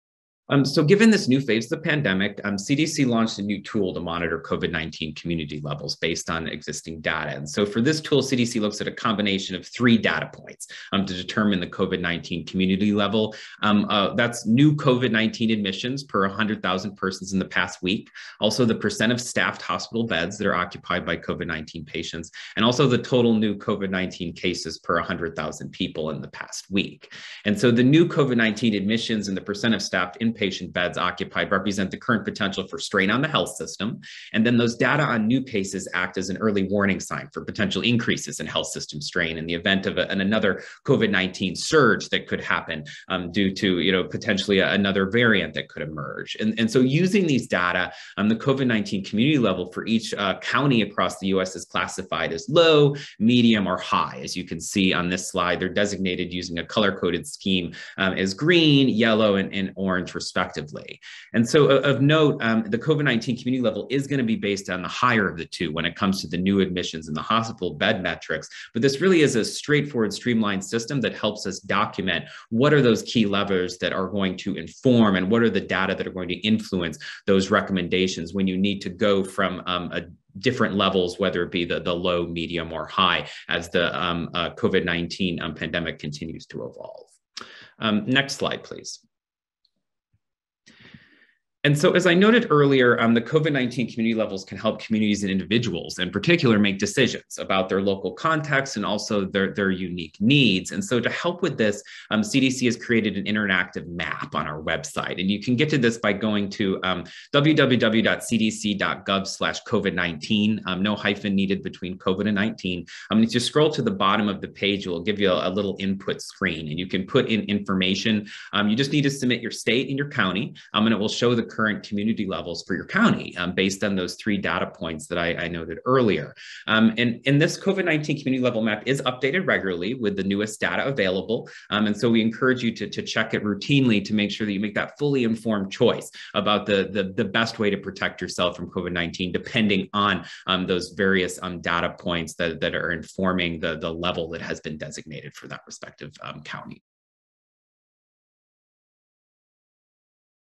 Explaining COVID-19 Community Levels
From CDC’s April 27, 2022, Webinar “Show me the data! How numbers affect COVID-19 communications,” this clip features Brian King, PhD, MPH, explaining how COVID-19 community levels help communities and individuals make informed decisions based on local context and specific needs.